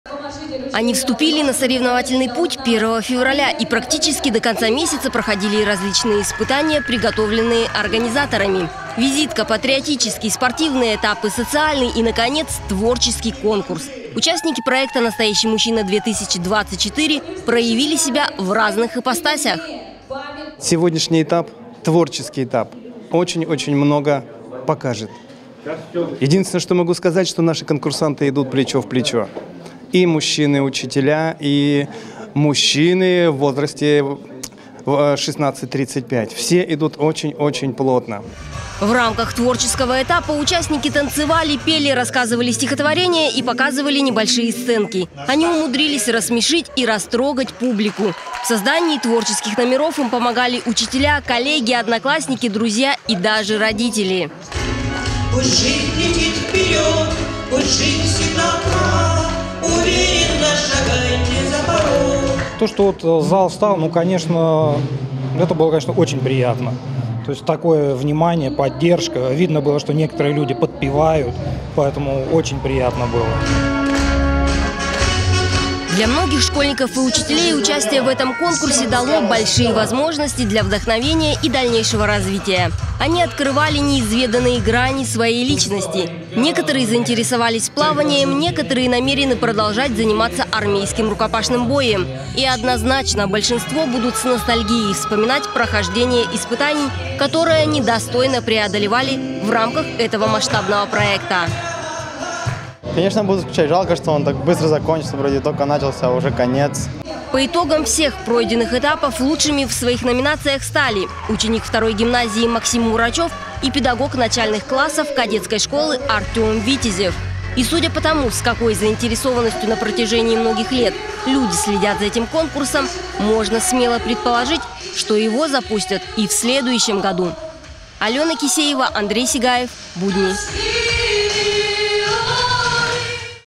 Победителей конкурса "Настоящий мужчина" определили в Бийске (22.02.24г., Бийское телевидение)
Трëхнедельный мужской марафон завершился. В Бийске определили победителей конкурса "Настоящий мужчина". Всё это время наша съёмочная группа присутствовала на каждом из соревновательных этапов. И в числе первых узнала, кто лучшие из лучших.

P.S.: Более подробную видеоверсию с заключительного этапа конкурса можно будет увидеть после праздников. Не пропустите новый выпуск программы "Я в Бийске", который выйдет 27 февраля.